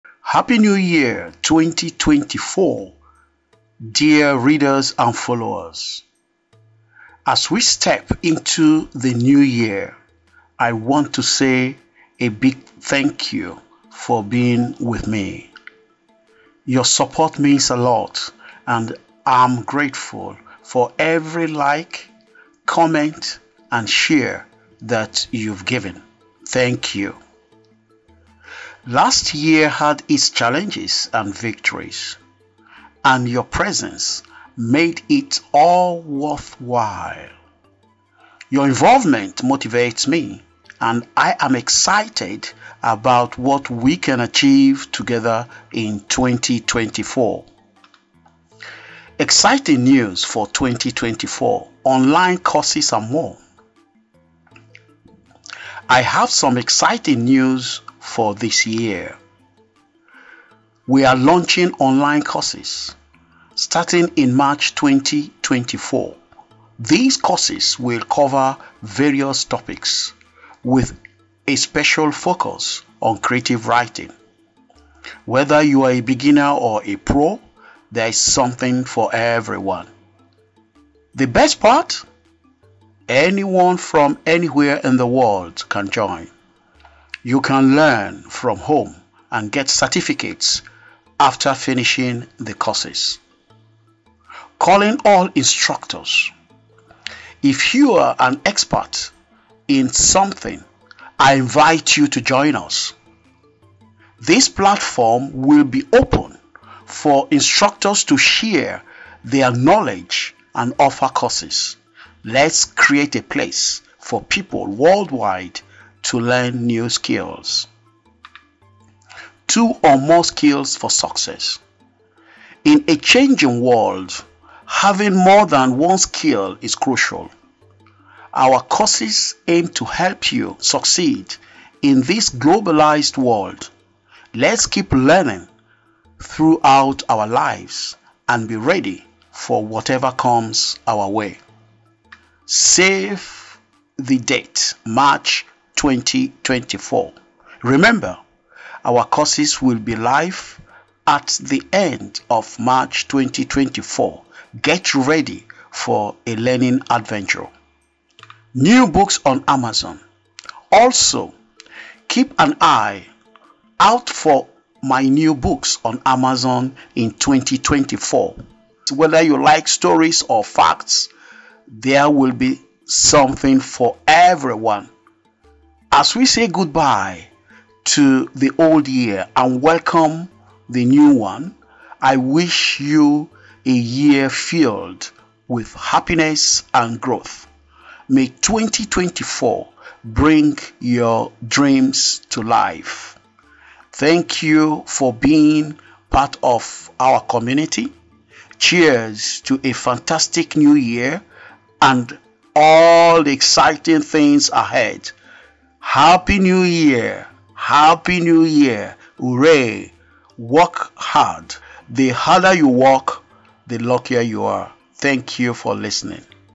Happy New Year Greetings and Announcements! | Learn Marketable Skills for Financial Freedom
Happy New Year Greetings🎉✨ Welcome to Nollywood France's New Year Celebration! 🌟 Join us for joyous greetings and exciting announcements as we usher in 2024! 🥳 Get ready for a game-changing revelation: Online Courses in Creative Writing and more! 📚🖋️ Unlock the key to financial freedom by learning two or more marketable skills. 🌐🏆 Save the date for our grand launch in March 2024! Happy New Year from Nollywood France! 🌍🎊